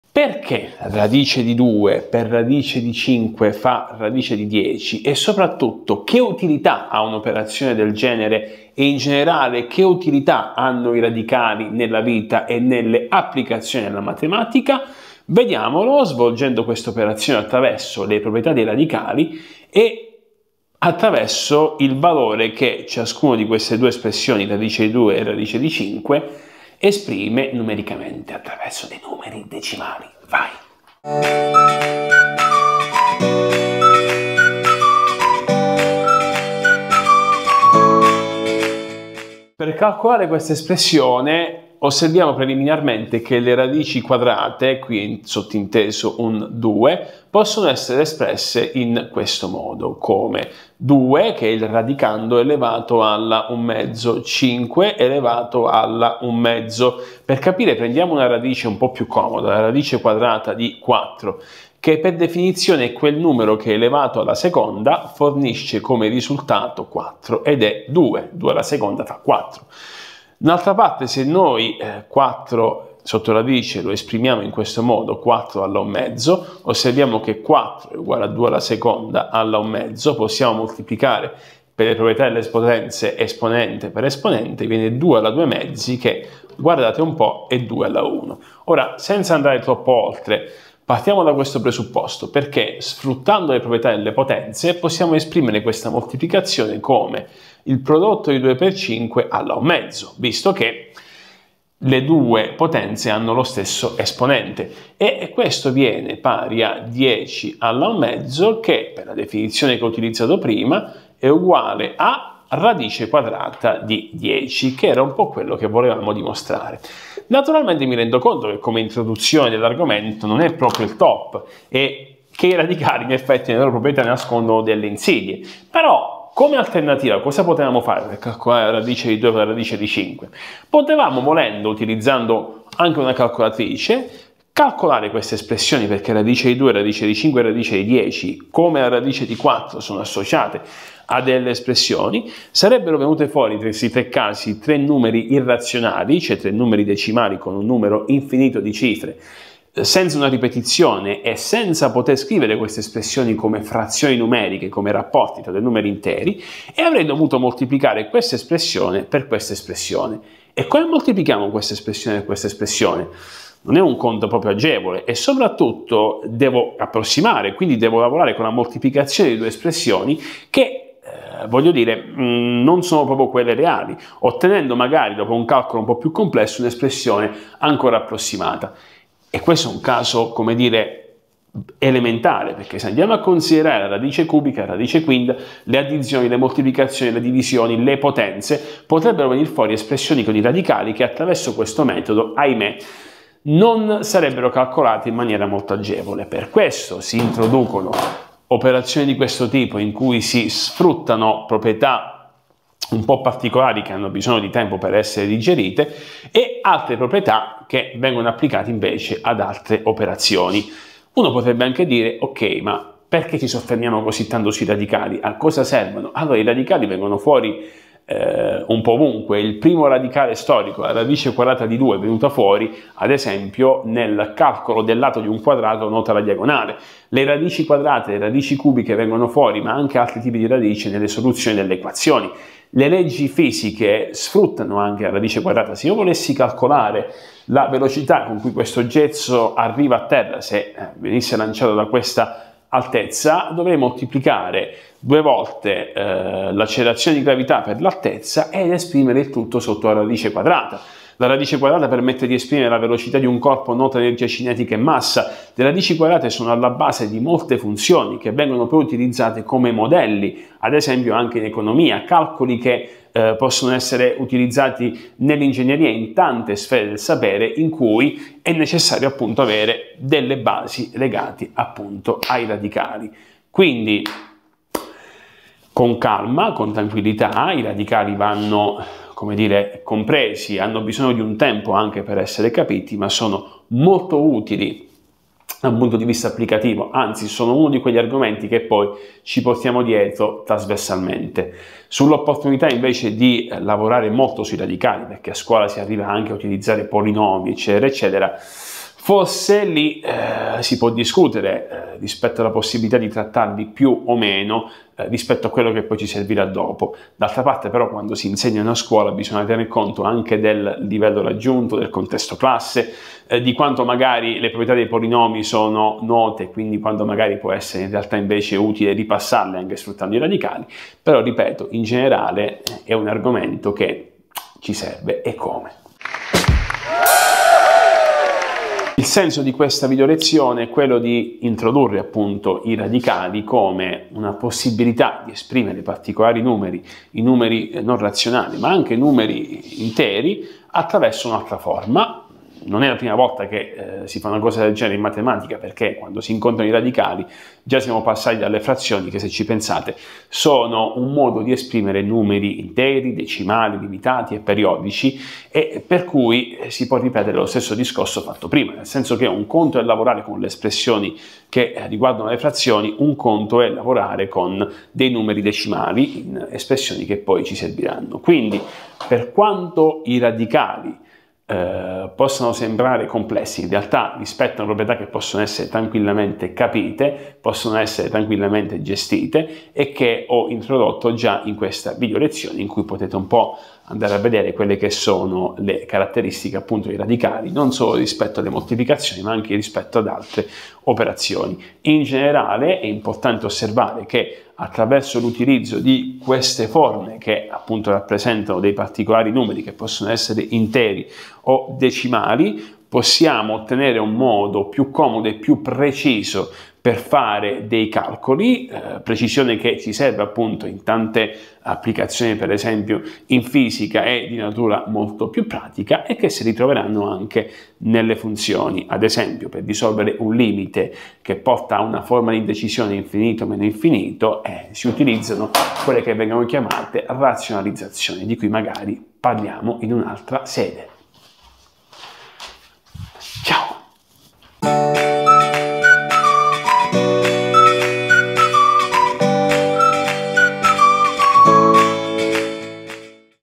A COSA SERVONO I RADICALI?
Video introduttivo sui radicali e sulla loro utilità in matematica e nelle applicazioni pratiche.
A cura del prof. Rocco Dedda.

Per info o curiosità puoi contattarmi